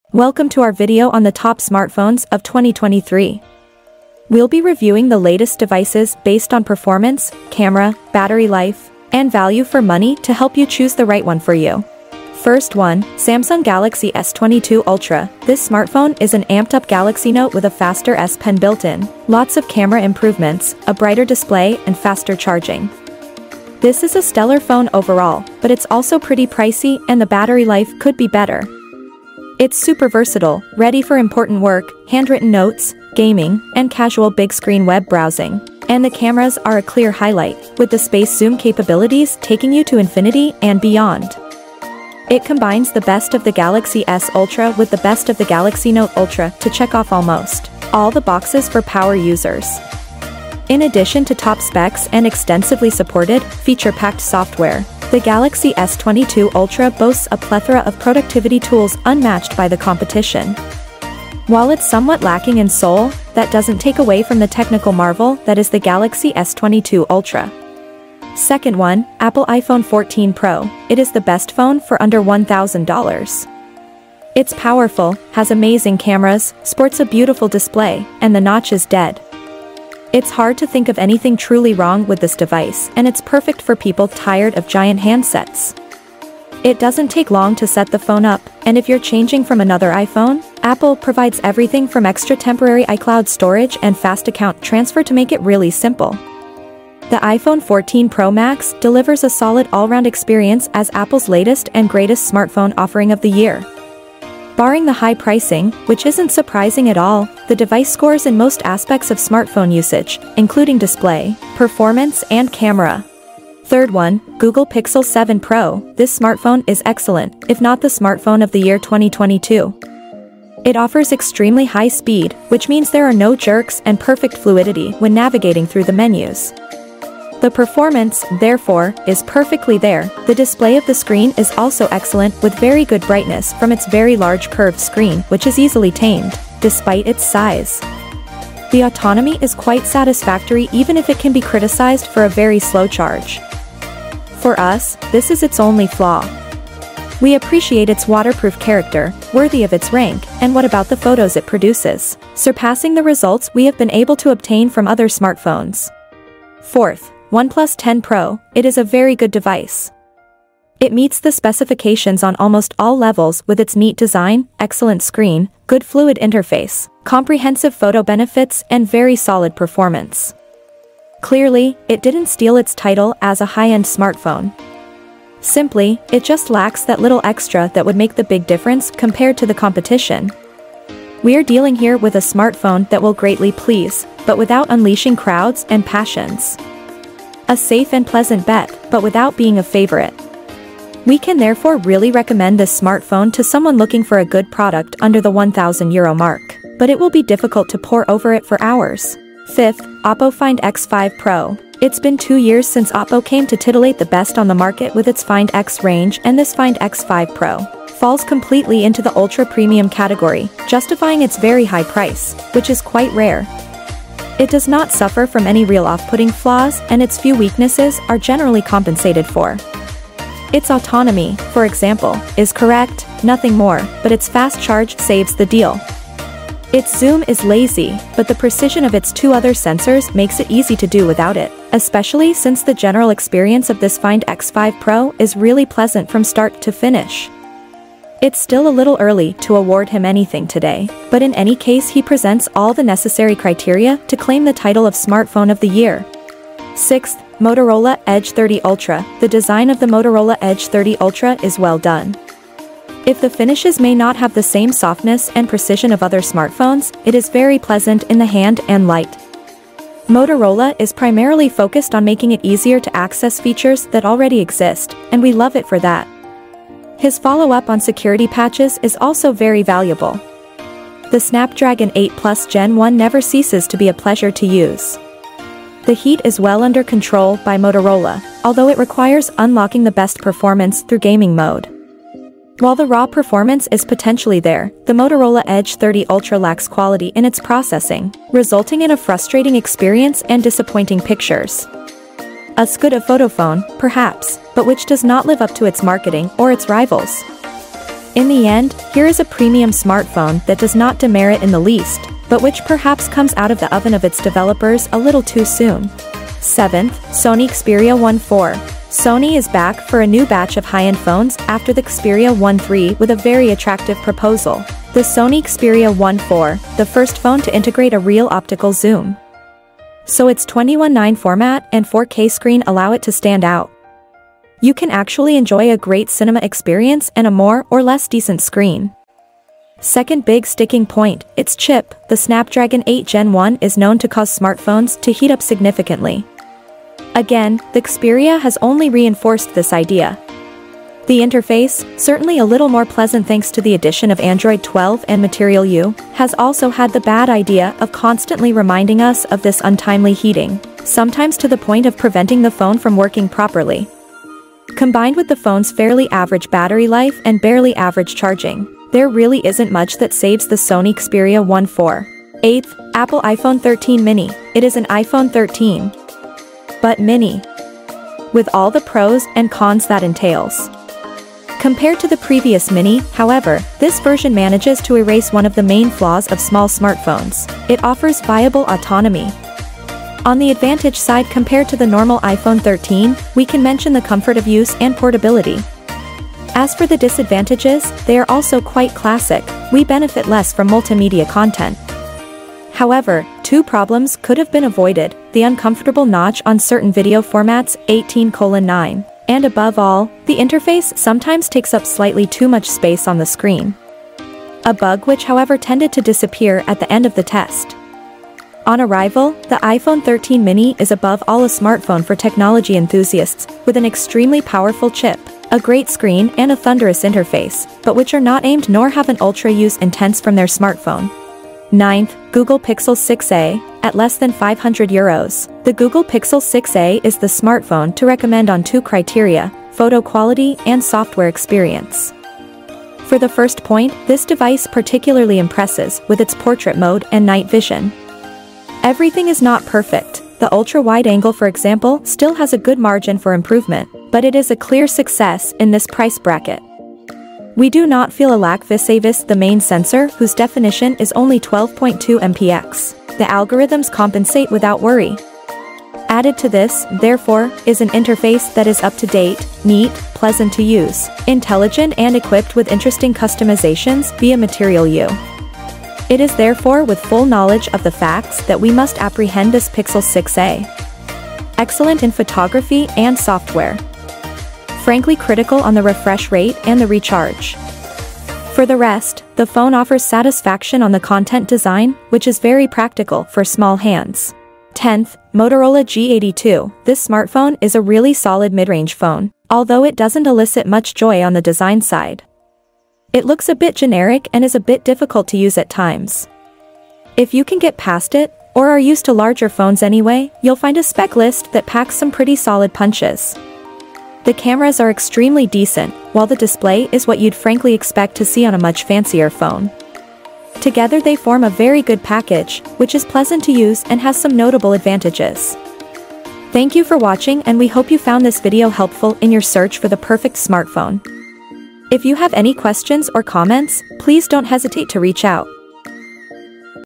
TOP 10 : Best Smartphones 2023
In this video, we present a listing of the Best Smartphones in 2023,

Links for details and characteristics :

Samsung Galaxy S22 Ultra
Apple iPhone 14 Pro
Google Pixel 7 Pro
OnePlus 10 Pro
Oppo Find X5 Pro
Motorola Edge 30 Ultra
Sony Xperia 1 IV
Apple iPhone 13 Mini
Google Pixel 6a
Motorola G82